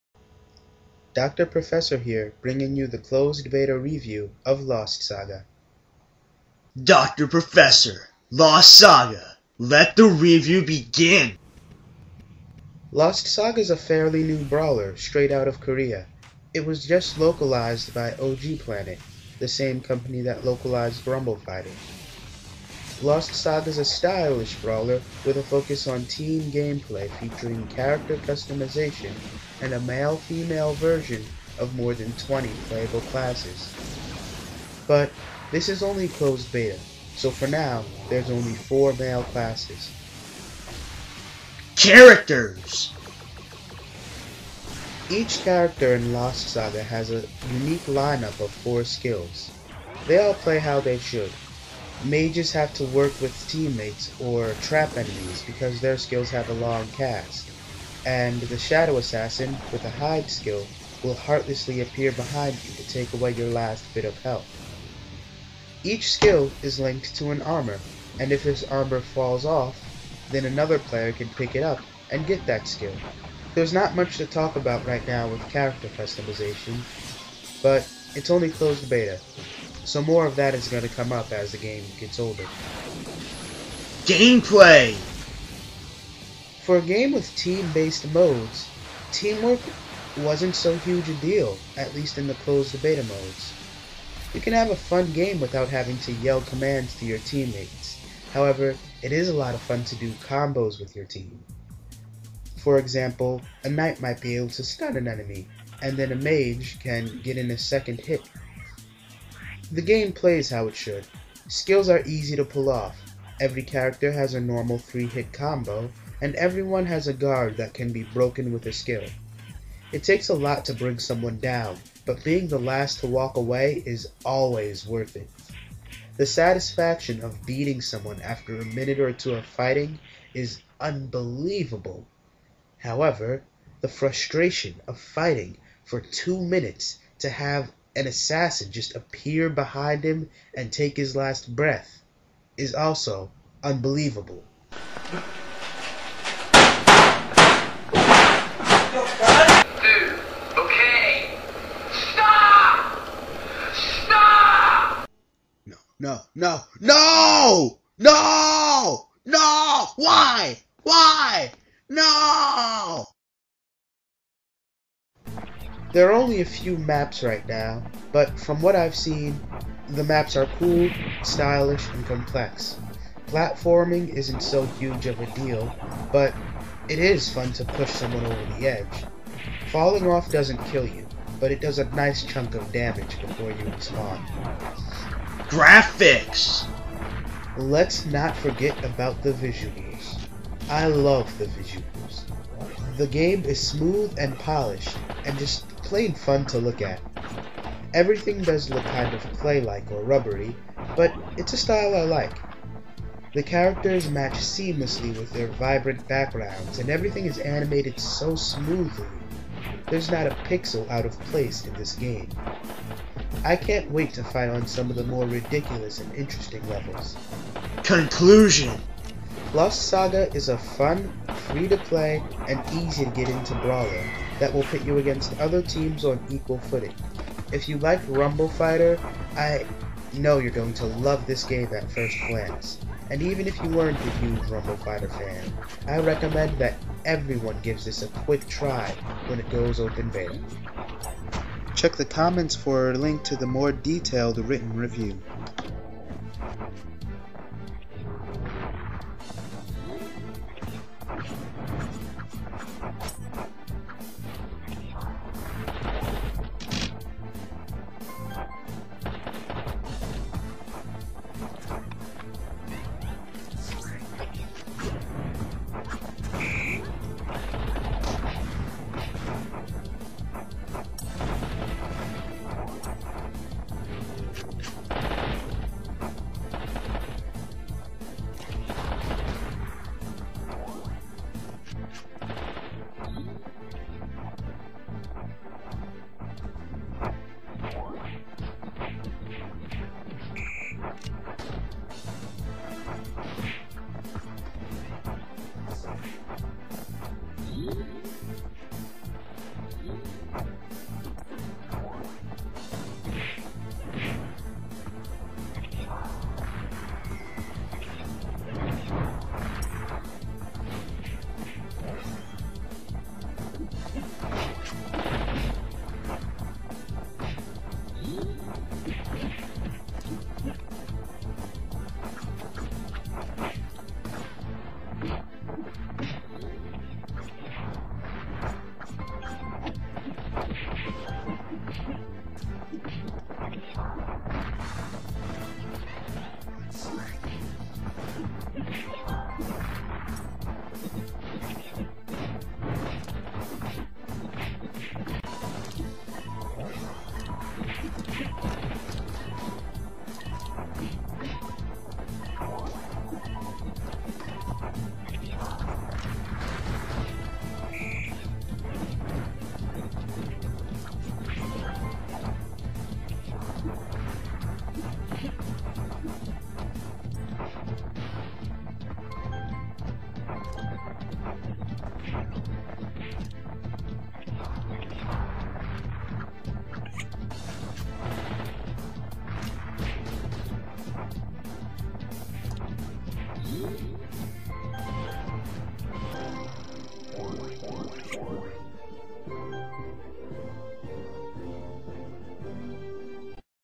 Lost Saga Review
Dr Professors take on the closed beta of Lost Saga.
Sorry, please wait for a full written review.